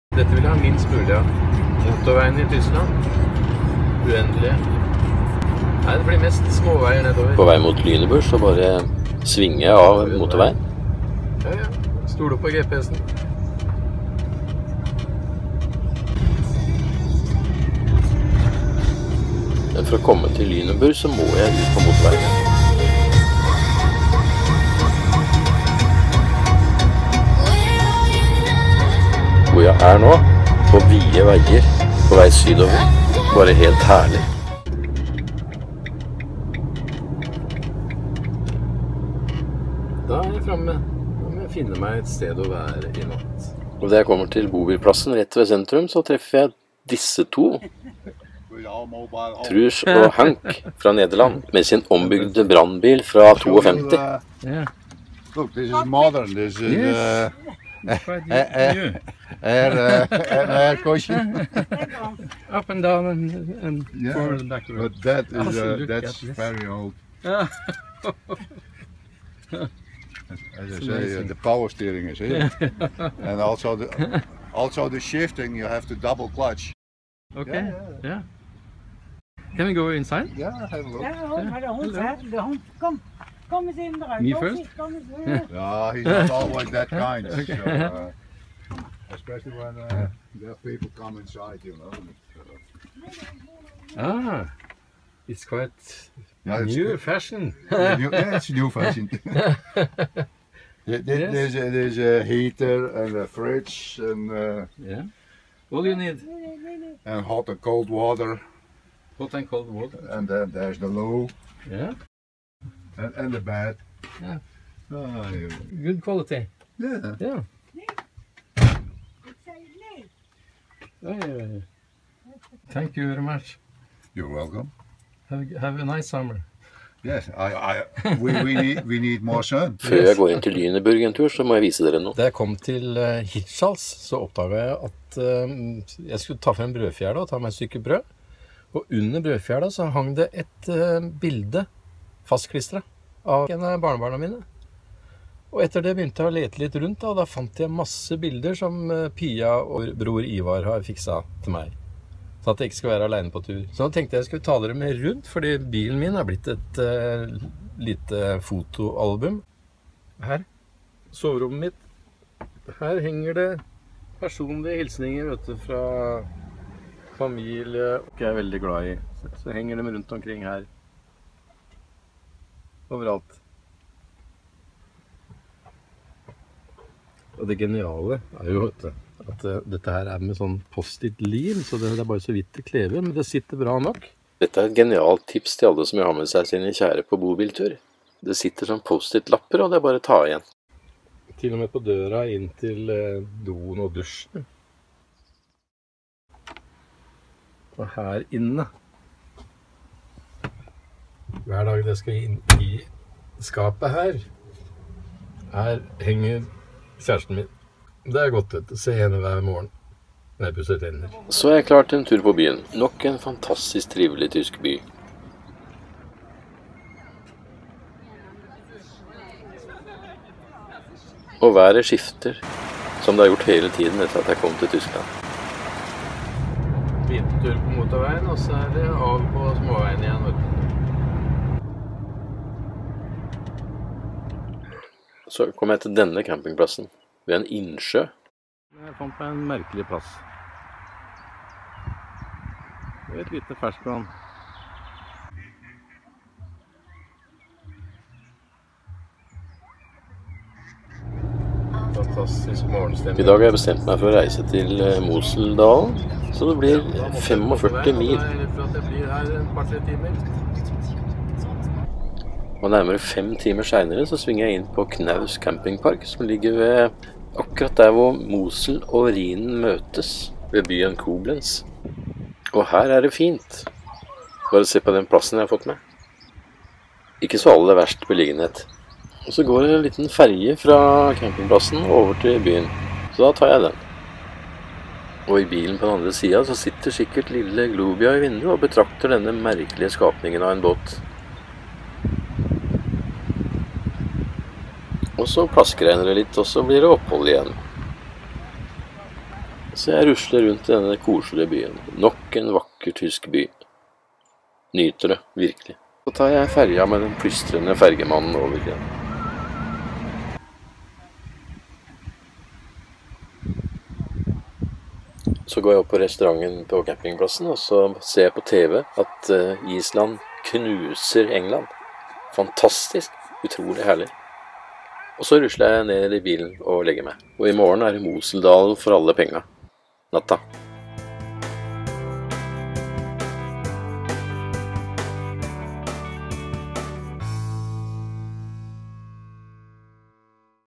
Europa 2016 Fra Lüneburg til Koblenz
Turen går videre fra Schleswig til Lüneburg for vår blogger Per Vie, som reiser med bobil rundt i Europa i hele sommer. Og fra Lüneburg blir det en liten tur innom Rotenburg før han setter kursen sydover mot Koblenz og Moseldalen. Følg hele turen på